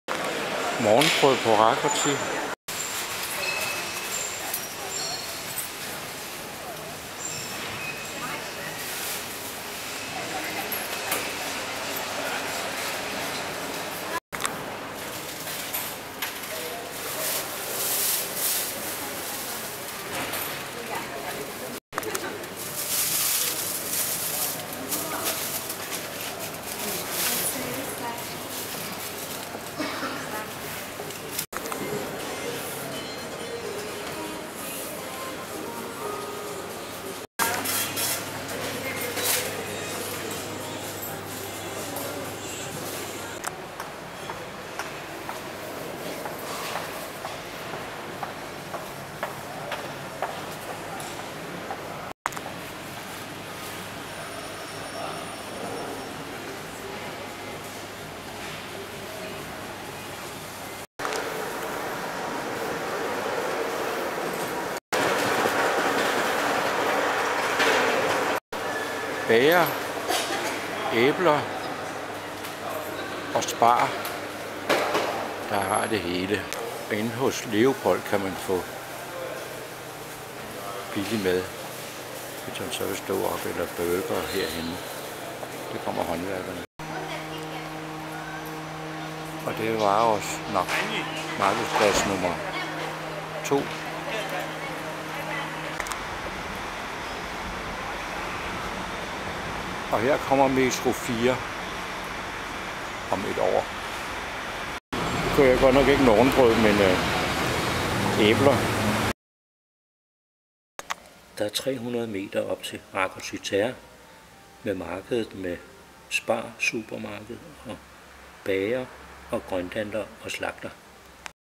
MARKET HALL / VASÁRCSARNOK RAKOCZI TÉR. BUDAPEST
okt. 2010
I MARKEDSHALLEN 5 MIN. GANG FRA KÖFARAGÓ FÅS FINT BAGERBRØD, ALLE GRØNTSAGER SPAR SUPERMARKED MED DET HELE OG FISK OG VILDTHANDLER MED LEVENDE FISK KARPER.